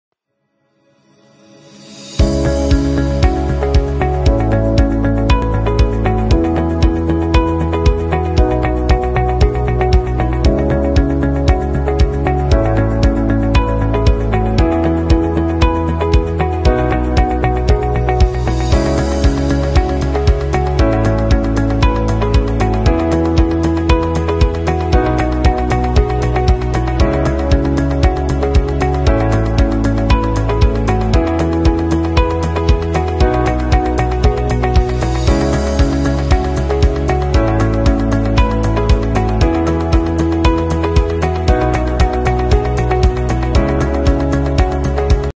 LCD Advertising Display 21.5''Automatic Sensor Hand Sanitizer Dispenser Kiosk Digital now available!
MUR99 VAT incl. : Price for the  Rental scheme « Tranquillité 24 mois ».

Contract and conditions available inside  « Downloads »  tab.

21.5" digital signage with automatic hand sanitizer dispenser.

Best places to be used: Hospitals, Meeting Rooms, Reception Desks, Hotel Lobbies, Schools, Train Stations, Airports, Toilets, Elevators, dining rooms, shopping malls, shops, etc...

It's industrial design with a full metal housing and trustworthy functions, its front side opens easily to refill the sanitizer with a large volume load of 3000ml.  The USB port is also located in front of the unit make it more convenient to change videos and photos. Also available for sale here.